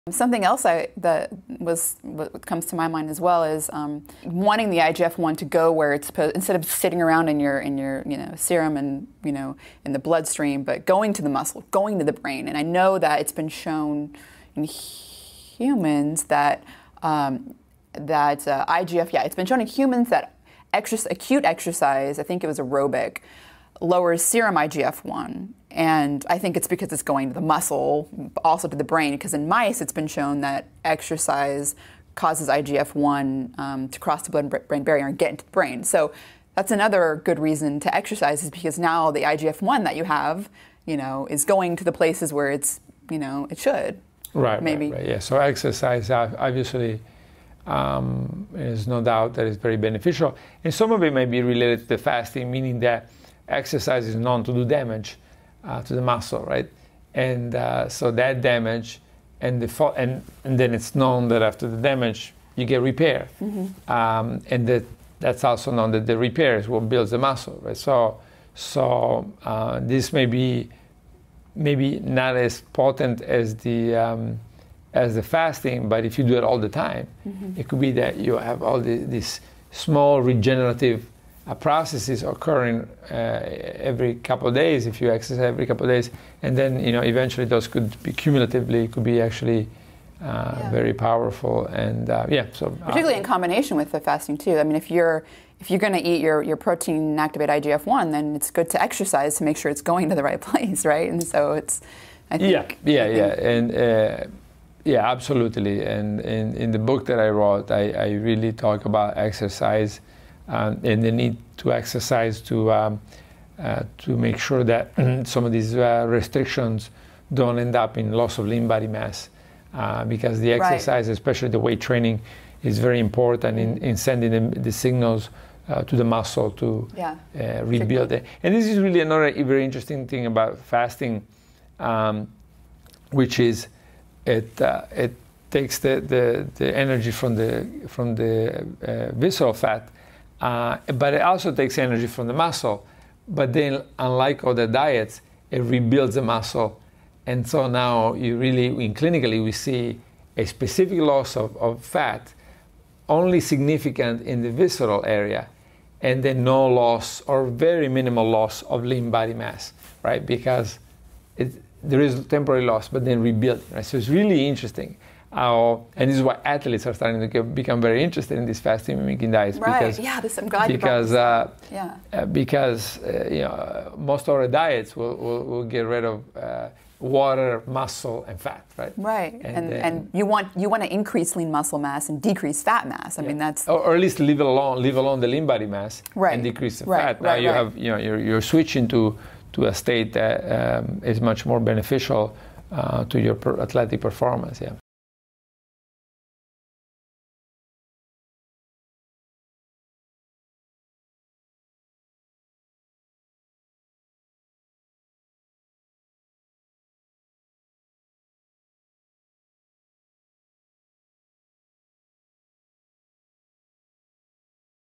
Does fasting cut fat or lean mass? | Valter Longo & Rhonda Patrick
Prolonged fasting and a fasting-mimicking diet are both comparable to exercise in that they involve cycles of cellular damage and repair. During a fasting-mimicking diet, the body consumes both visceral fat and lean body mass for energy. However, upon refeeding only lean muscle mass is regenerated. The overall result, a loss of visceral fat without depleting lean muscle mass, is a desirable feature of a prolonged fast, particularly for athletic performance. In this clip, Dr. Valter Longo describes how a fasting-mimicking diet can be used for fat loss while preserving lean muscle mass.

Original episode published on Oct 1, 2016